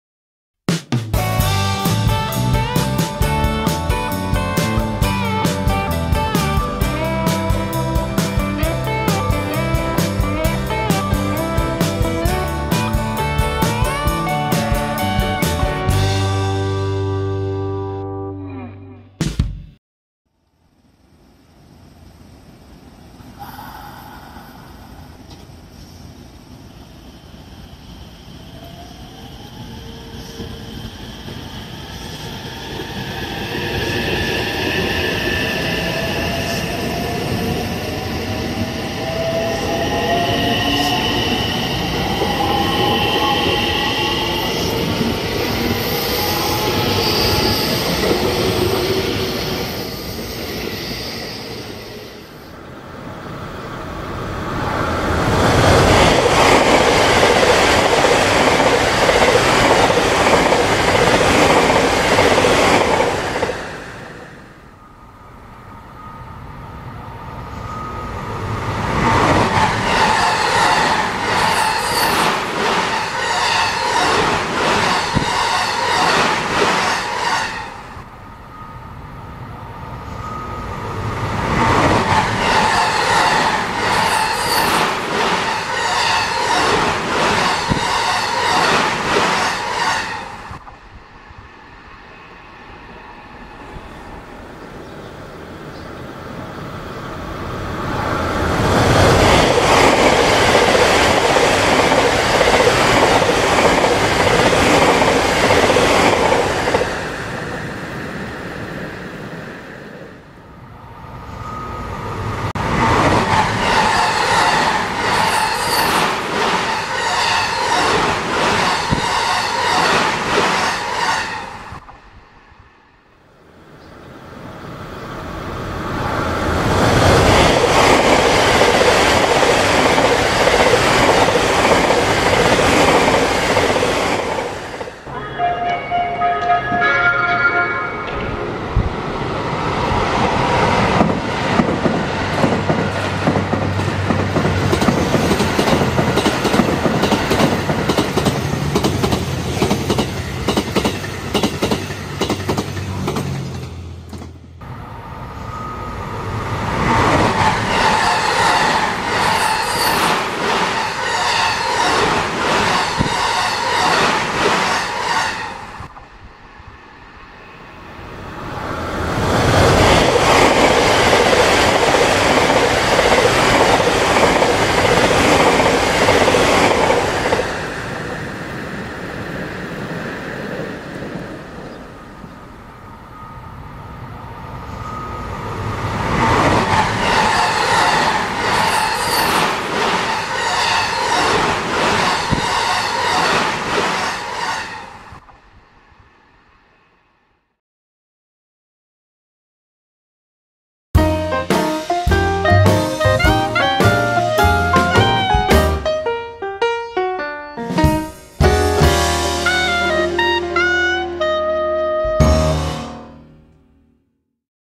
Nゲージ鉄道模型 【225-100+223-2000 新快速】
225-100と223-2000の混合編成の走行動画です